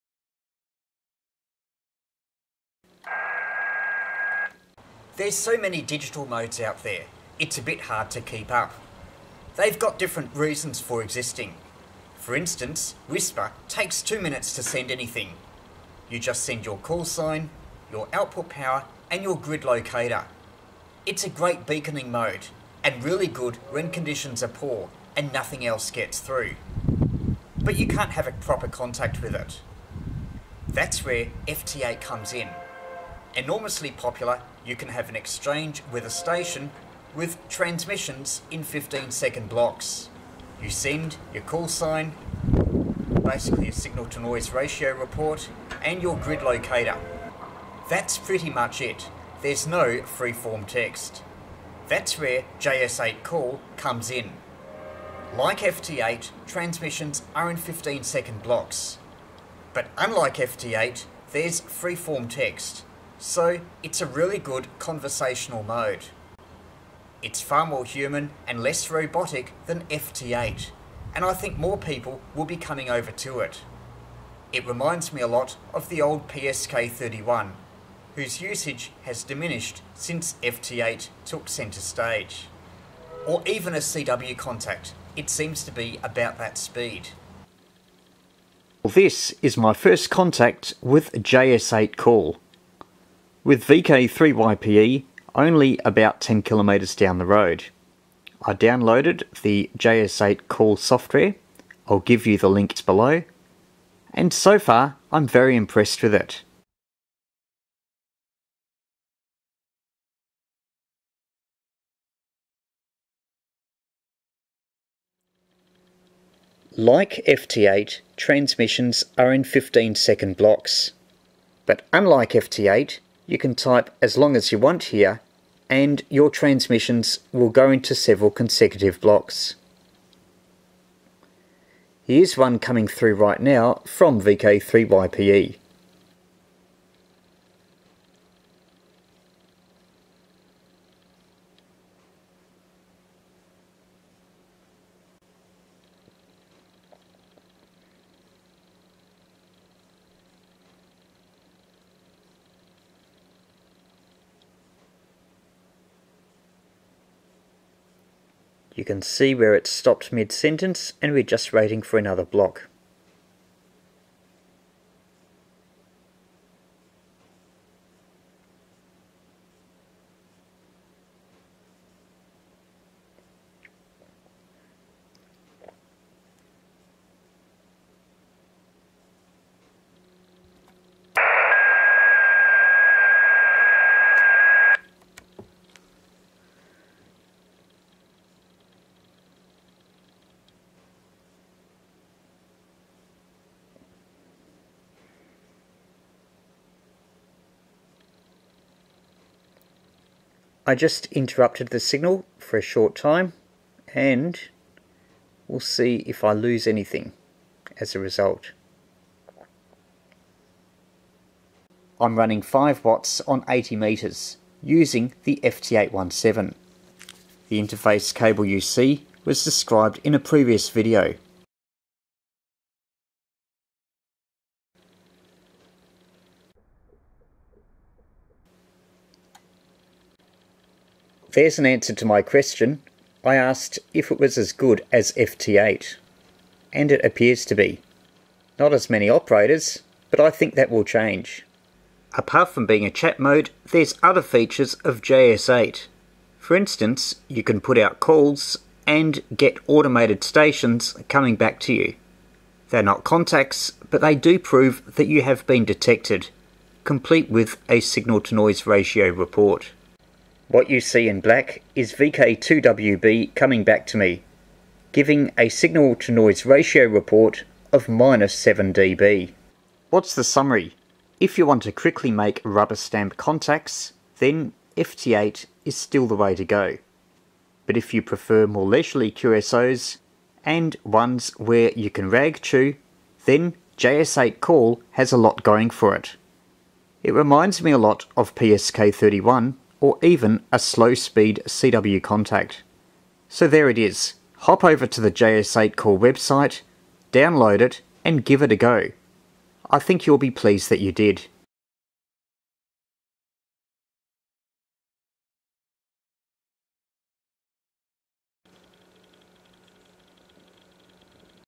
JS8: My first contact
My first contact with the JS8 digital mode on 80m. Like FT8 its transmissions are in 15 second blocks. But unlike FT8 it's a good chat mode ideal for ragchewing (a bit like PSK31 or CW).